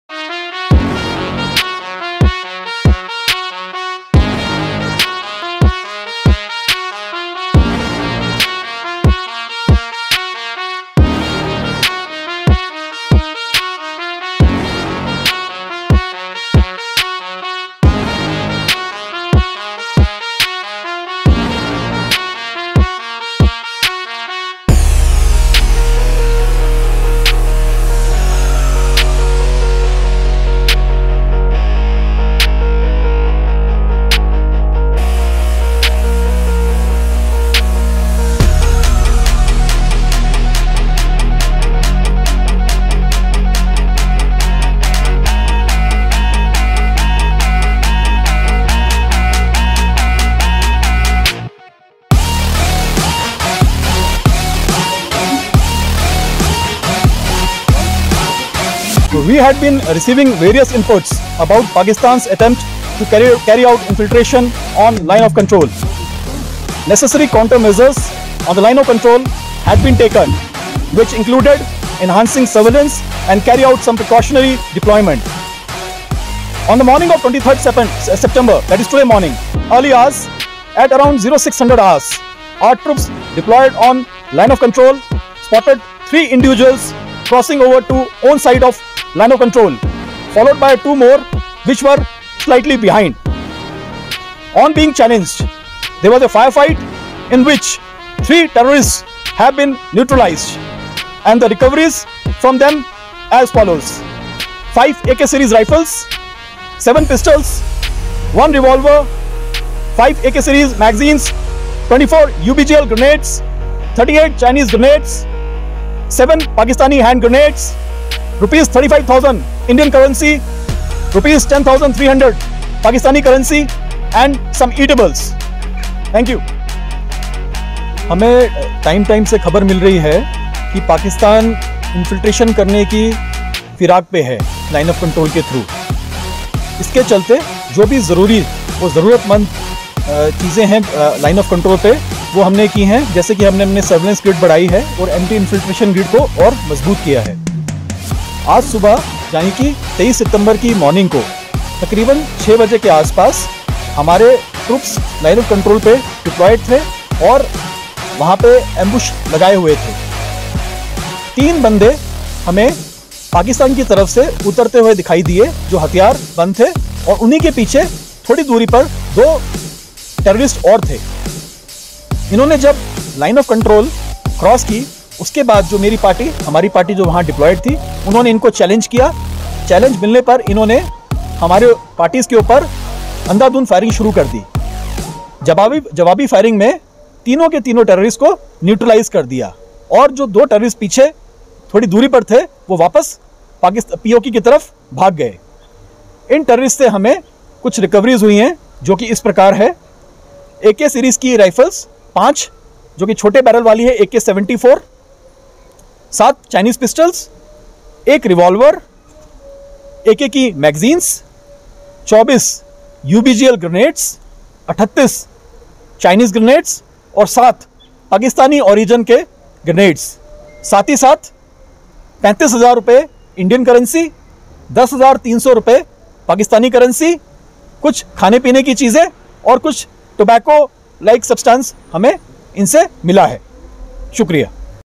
LoC , Uri Sect | 3 PakTerrorists To 72Hoors Ft. GOC,19InfantryDivision Virendra Vats Sir ! TJH🇮🇳🙏🔥🥳
3 Militants Killed In Uri Baramulla

5 AKs, 8 Pistols, 45 Grenades, 2 UBGL & Huge Ammunition Recovered

September 23: General Officer Commanding (GOC) 19 Infantry Division Virendra Vats on Thursday said that millitants killed along the Line Of control (LOC) in Uri sector were carrying short guns being used in the civilian killings.

Addressing a press conference at Rampur brigade as per Kashmir Despatch, GOC said that heavily armed militants were carrying short guns which they are using in civilian killings.

The top army officer said that this group was likely coming to supply arms and ammunition this side to support the militancy in Kashmir.

Pertinently, army foiled a major infiltration bid along the line of control (LOC) in Rampur sector of Uri and killed three heavily armed militants.